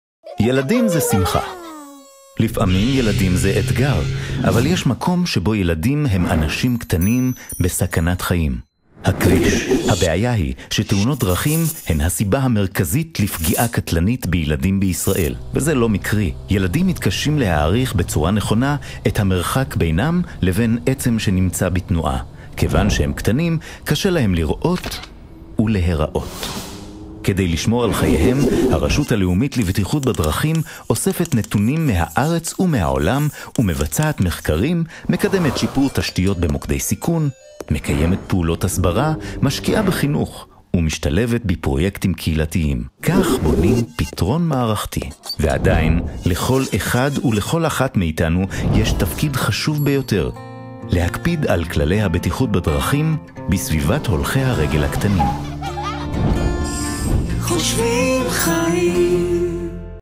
פרויקט "דקה ל-20:00" - בטיחות ילדים בכבישים
לפני כחודש התחלנו בפעילות חדשה ב"דקה ל-20:00" עבור הרשות הלאומית לבטיחות בדרכים.
בואו לצפות בסרטון השני שהופק במסגרת הפרויקט, העוסק בהתנהגות ילדים ובטיחותם בסביבת הכביש.